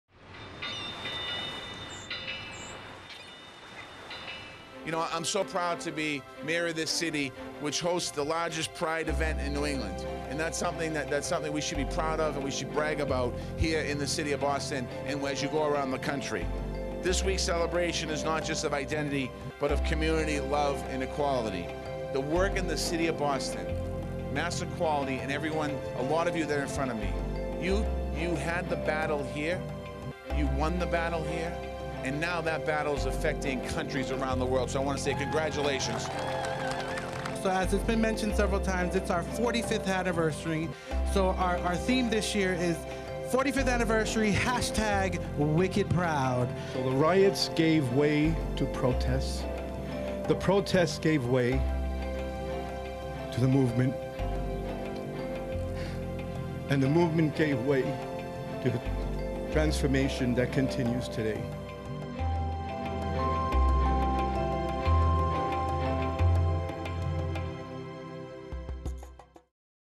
Boston Pride Flag Raising Celebration 2015
Mayor Walsh joins elected officials, Boston Pride members, community leaders and the public to kick off Pride week in Boston with a Pride Flag Raising ceremony on Boston City Hall Plaza.